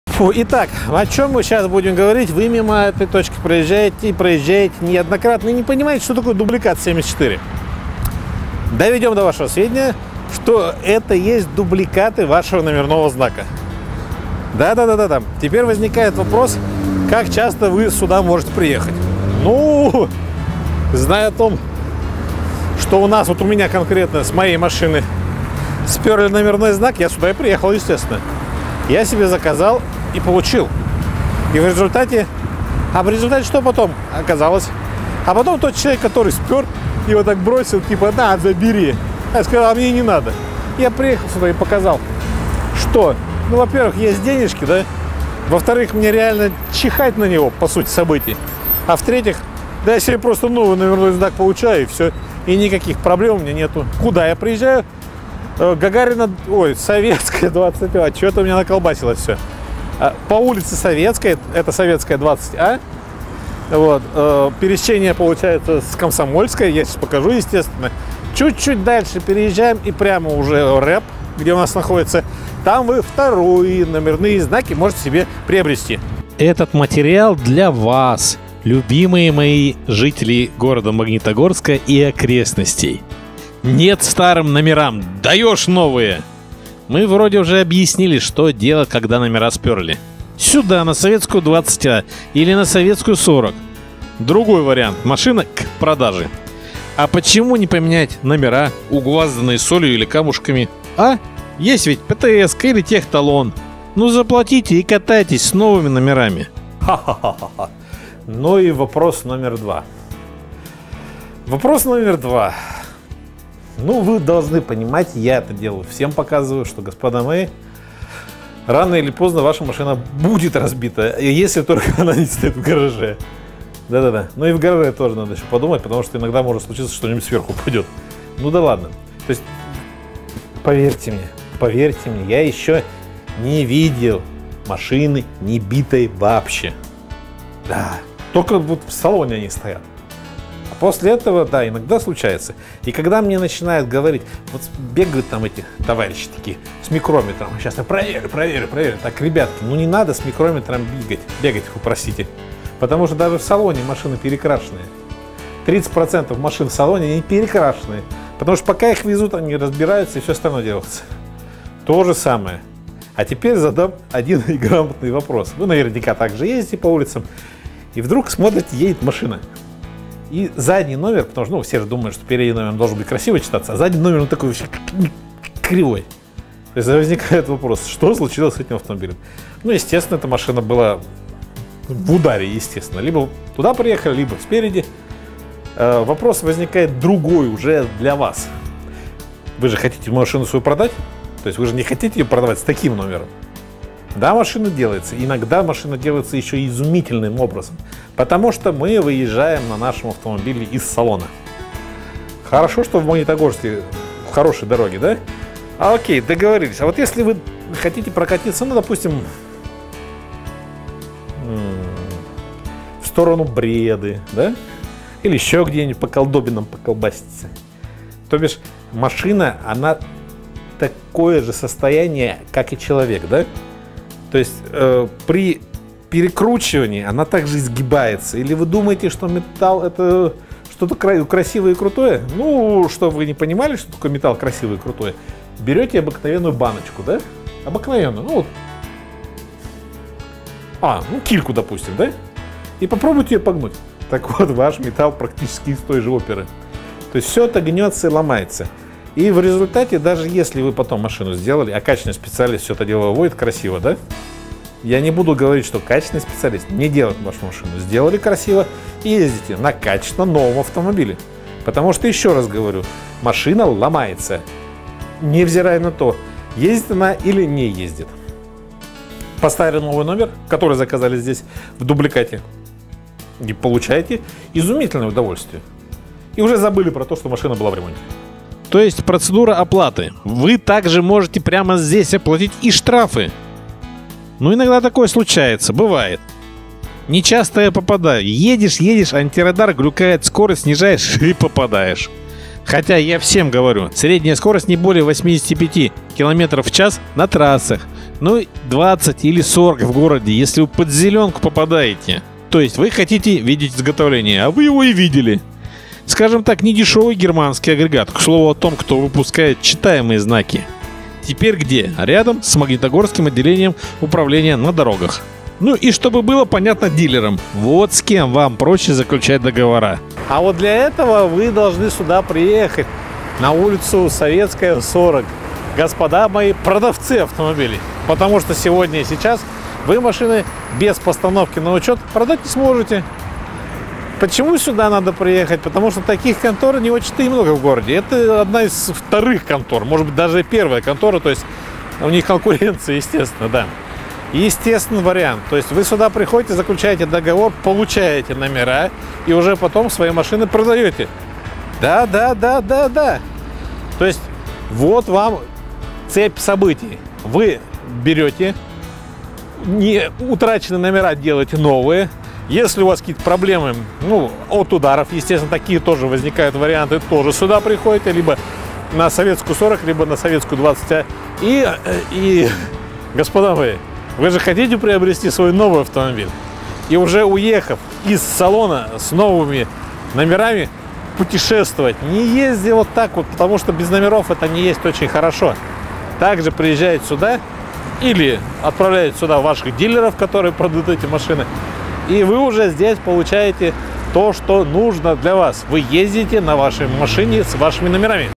Dublikat 74 11 19
Алексей Челейкин - изготовление номеров в Магнитогорске от фирмы Дубликат 74 11 19, Alexey Cheleykin - new car nomber by Dublikat 74 11 19